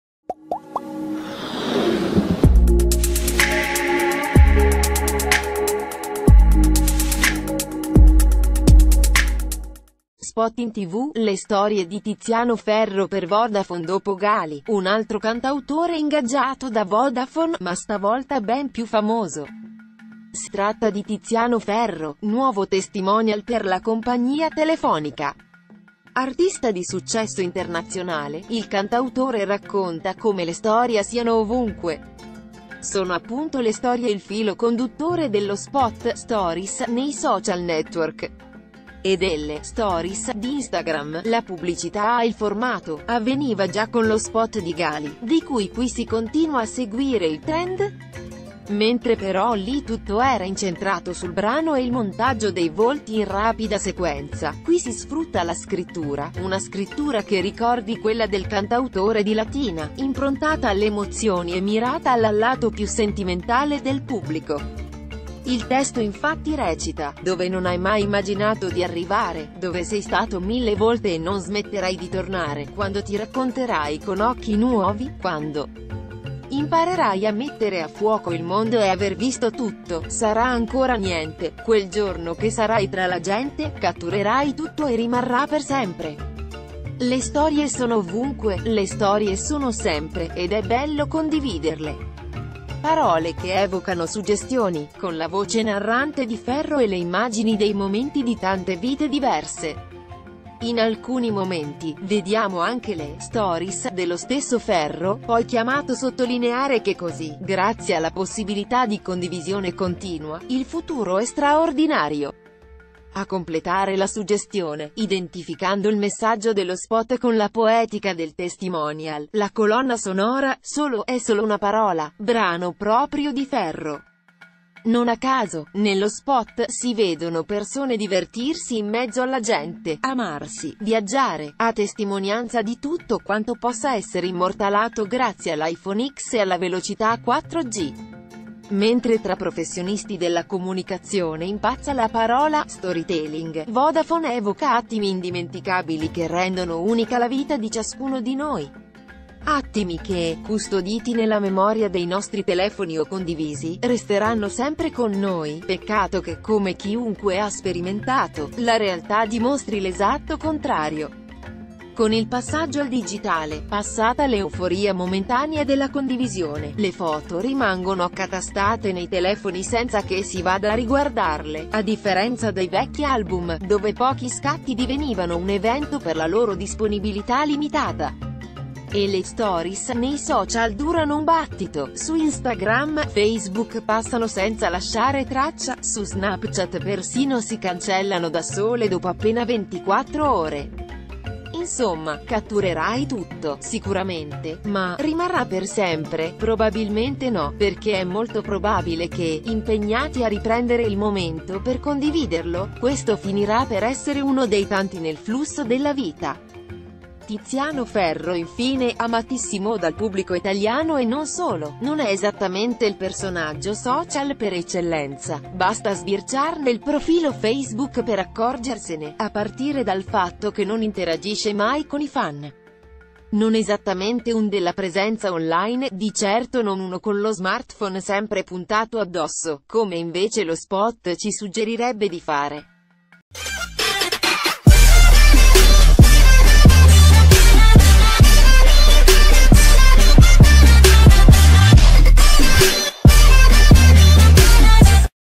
Spot in tv: le storie di Tiziano Ferro per Vodafone
Spot in tv: le storie di Tiziano Ferro per Vodafone . Spot in tv: le storie di Tiziano Ferro per Vodafone Dopo Ghali, un altro cantautore ingaggiato da Vodafone: ma stavolta ben più famoso. Si tratta di Tiziano Ferro, nuovo testimonial per la compagnia t... ...